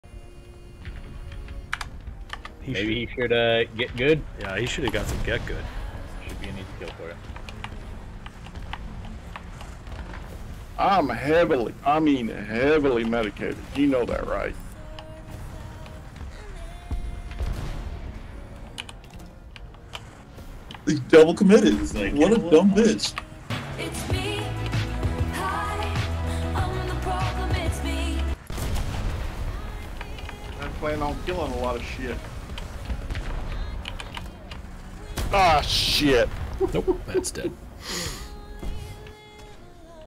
1 shot 2 Kill - Short
These feel rare, but a double kill in a FRENCH LIGHT?!